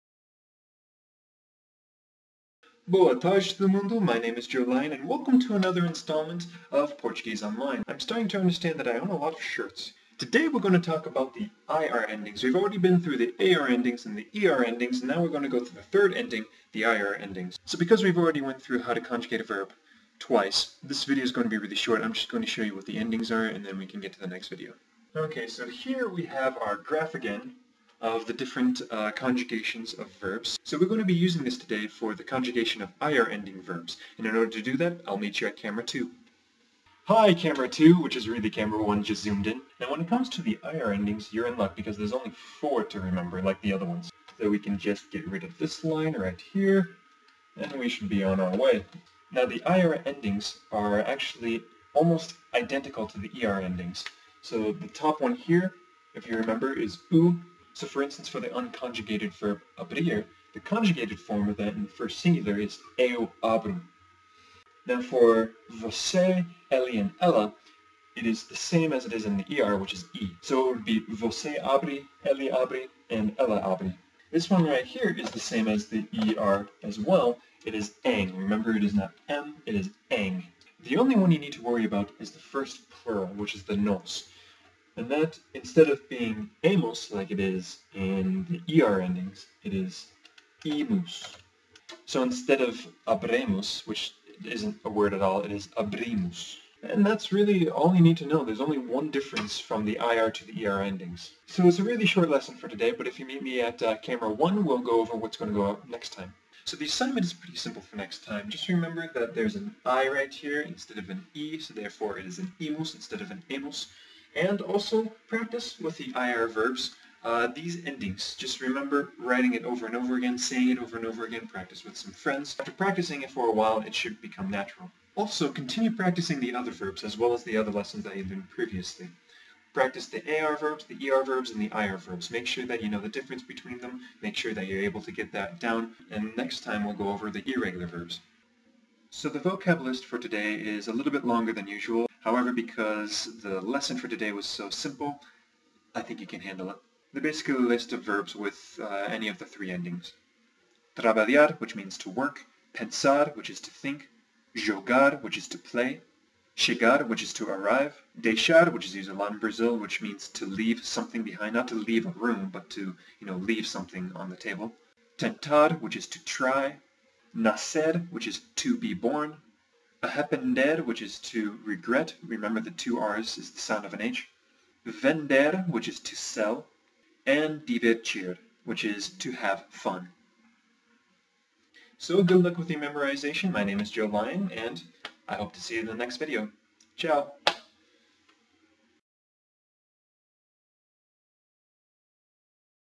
Portuguese Online 8- ir endings
Online Portuguese Lessons. This short Episode finishes the regular verb endings with -ir ending verbs.